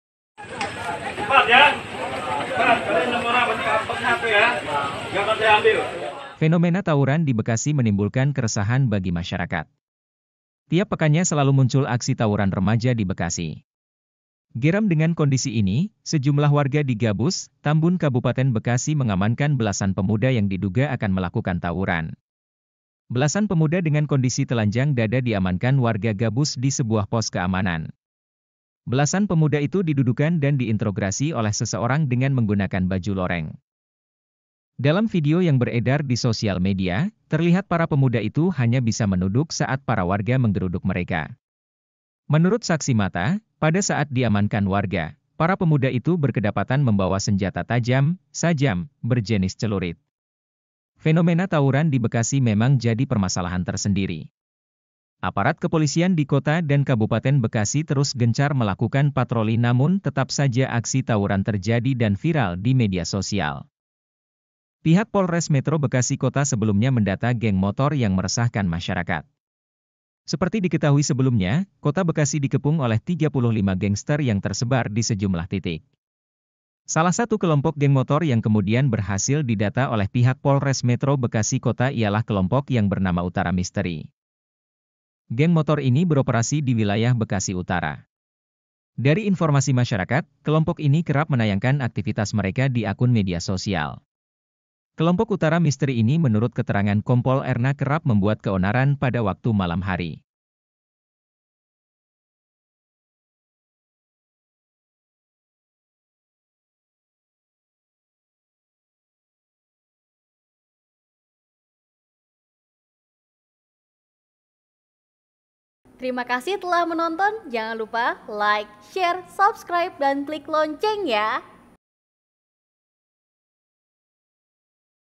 🔴 Warga Gabus Bekasi Amankan Belasan Pemuda Hendak Tawuran
Fenomena tawuran di Bekasi menimbulkan keresahan bagi masyarakat. Tiap pekannya selalu muncul aksi tawuran remaja di Bekasi.

Geram dengan kondisi ini, sejumlah warga di Gabus, Tambun Kabupaten Bekasi mengamankan belasan pemuda yang diduga akan melakukan tawuran.

Belasan pemuda dengan kondisi telanjang dada diamankan warga Gabus di sebuah pos keamanan. Belasan pemuda itu didudukan dan diintrograsi oleh seseorang dengan menggunakan baju loreng.

Menurut saksi mata, pada saat diamankan warga, para pemuda itu berkedapatan membawa senjata tajam (sajam) berjenis celurit.

Pihak Polres Metro Bekasi Kota sebelumnya mendata geng motor yang meresahkan masyarakat. Seperti diketahui sebelumnya, Kota Bekasi dikepung oleh 35 gangster yang tersebar di sejumlah titik.

Salah satu kelompok geng motor yang kemudian berhasil didata oleh pihak Polres Metro Bekasi Kota ialah kelompok yang bernama Utara Misteri.

Kelompok Utara Misteri ini menurut keterangan Kompol Erna kerap membuat keonaran pada waktu malam hari.